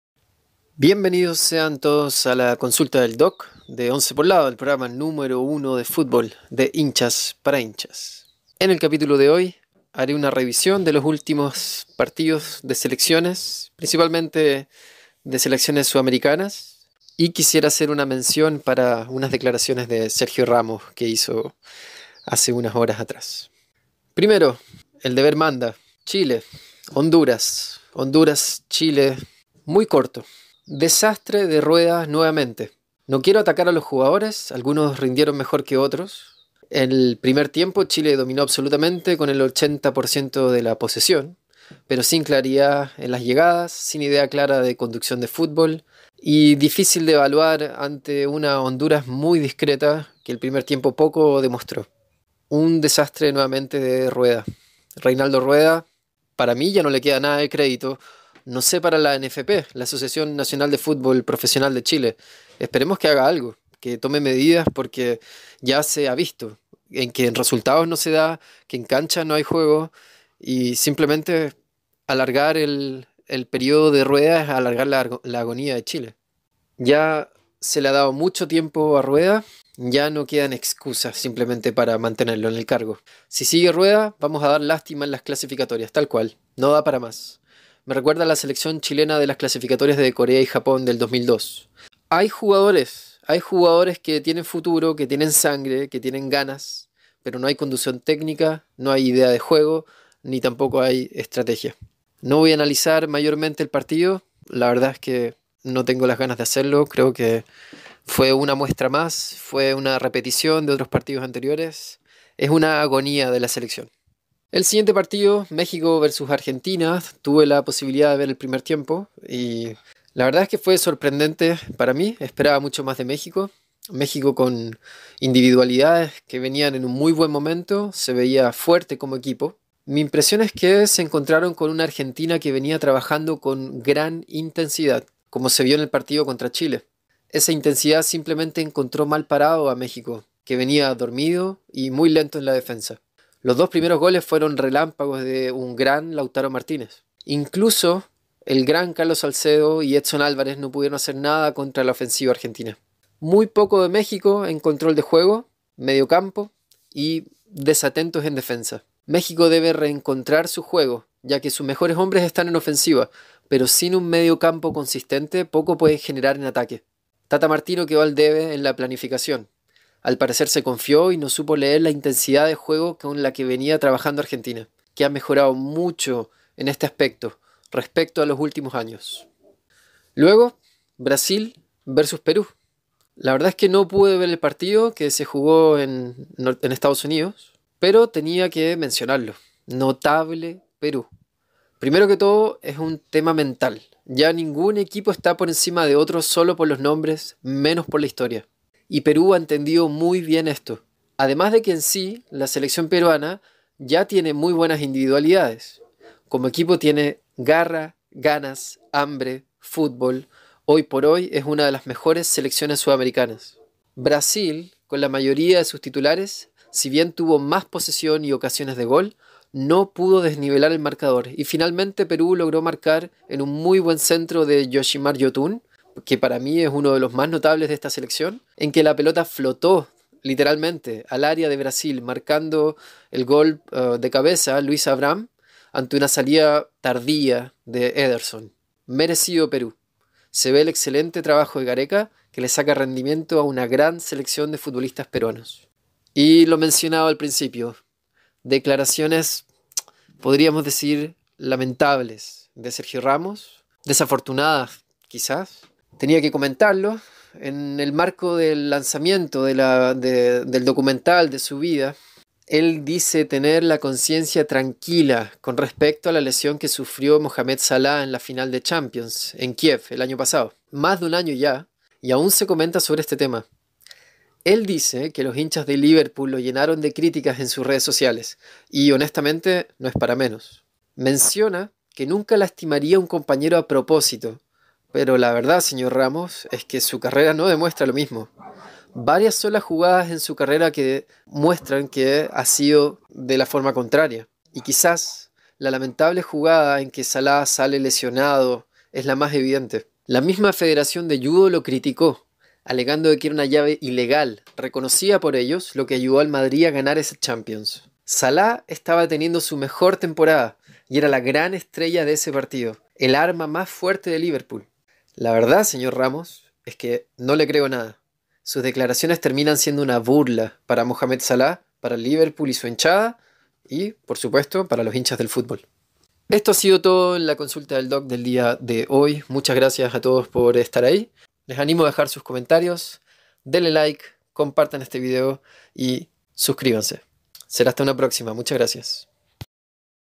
CHILE Y MÉXICO PIERDEN / PERÚ NOTABLE / RAMOS SIN VERGÜENZA - La Consulta del Doc
Comentarios sobre la última fecha de fútbol de selecciones, y una breve mención a las últimas declaraciones de Sergio Ramos sobre la polémica jugada en que Mo Salah sale lesionado en la final de Champions 2018.